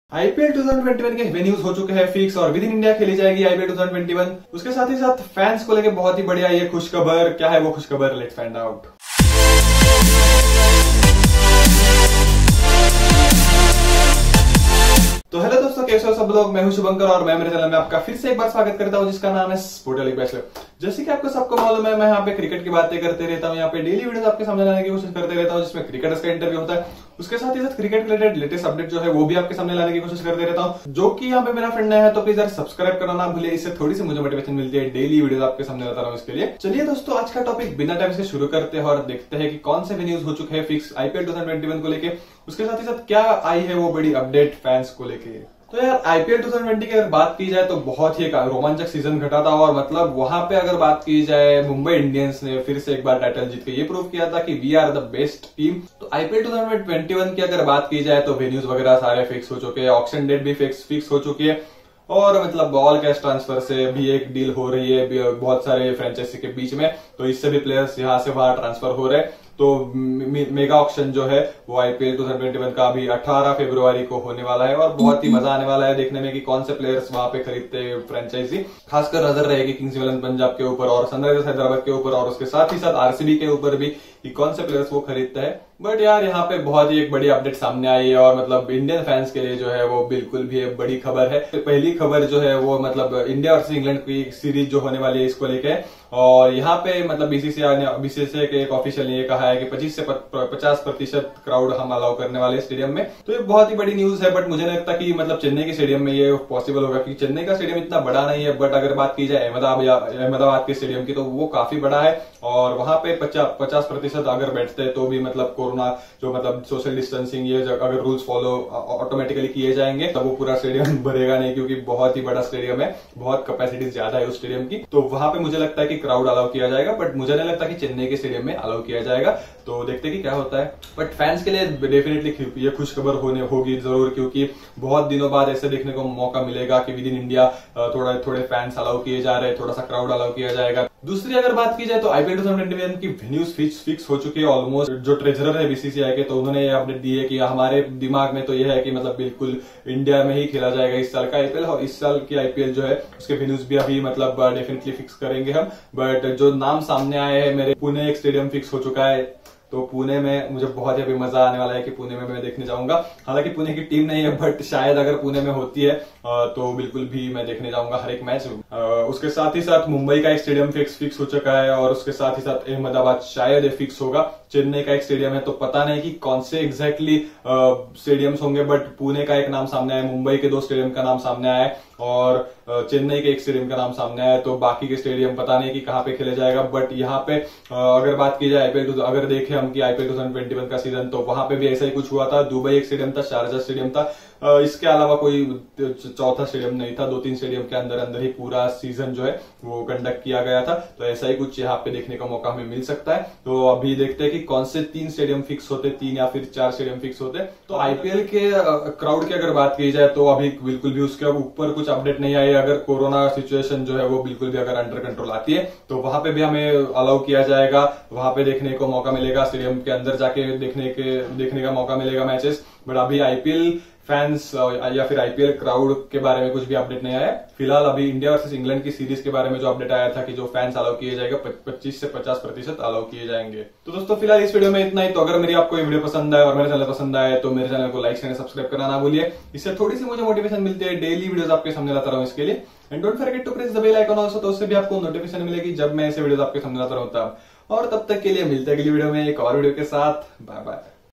IPL 2021 VENUE UPDATE | IPL 2021 is going to take place in INDIA itself| IPL 2021 NEWS
Welcome to Sportaholic Bachelor. In today's episode of sportaholic bachelor , we have some awesome news for the cricket fans like you. there are two updates one is related to IPL venues and second is related to India vs England Series. Watch the video till end .